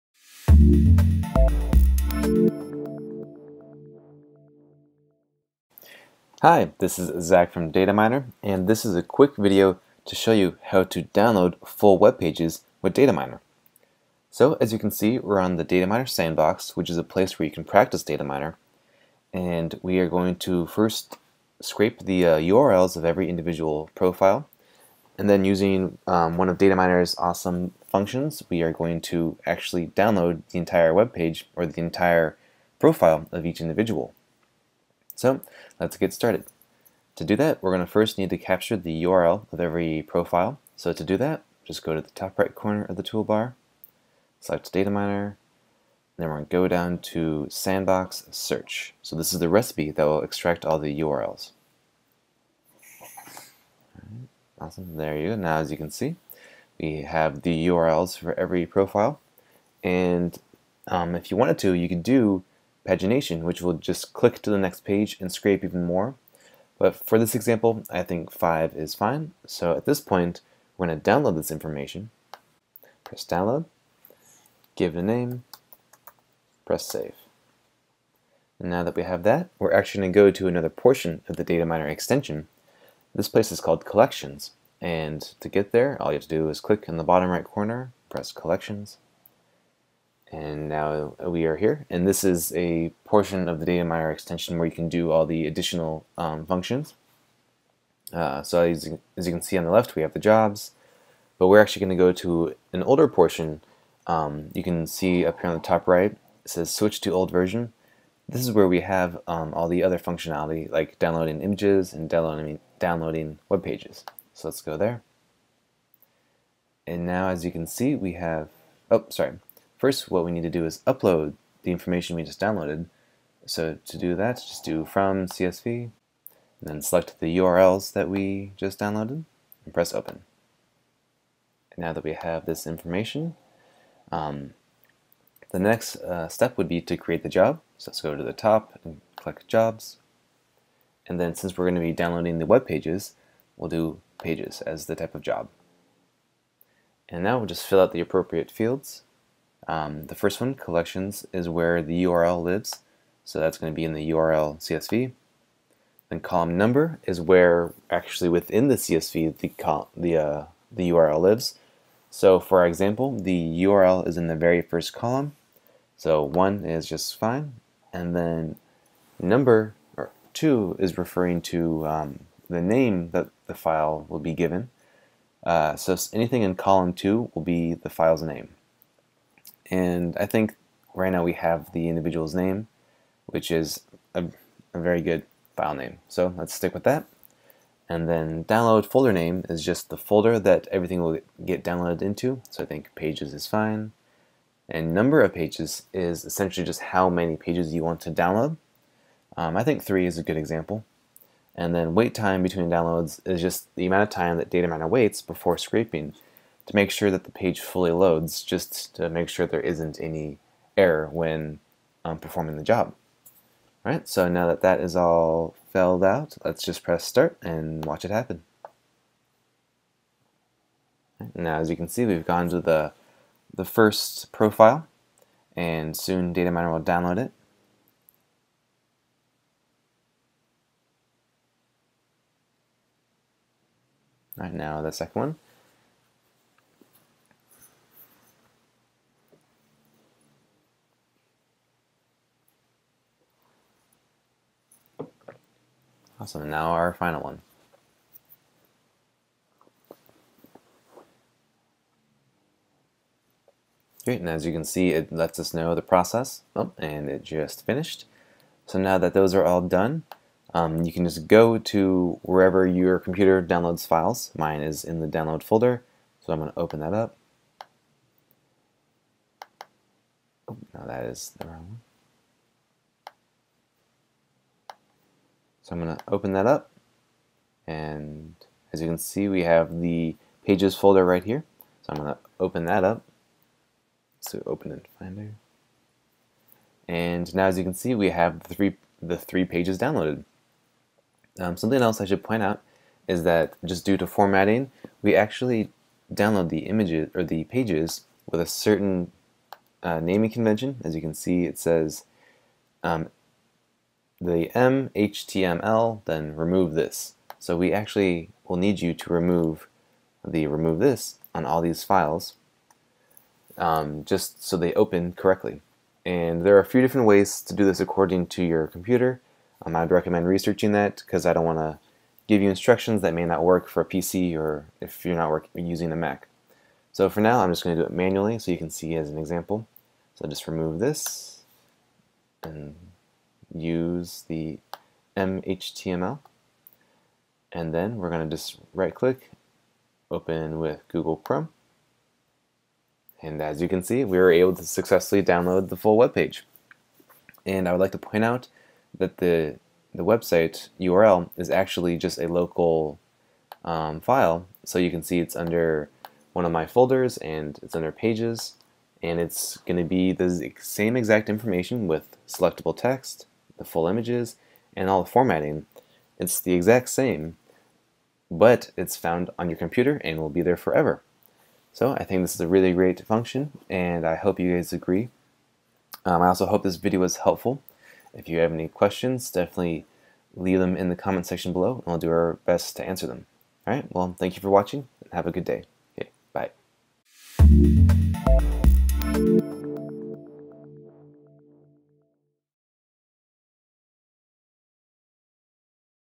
Download full web pages with Data Miner
This video will quickly cover the process of downloading full web pages with the Data Miner tool. This is will capture text, images and any other page elements, without even needing a recipe for the site. (Temporarily Unavailable)

Also, try our new tool Recipe Creator! Create your own custom recipes in minutes.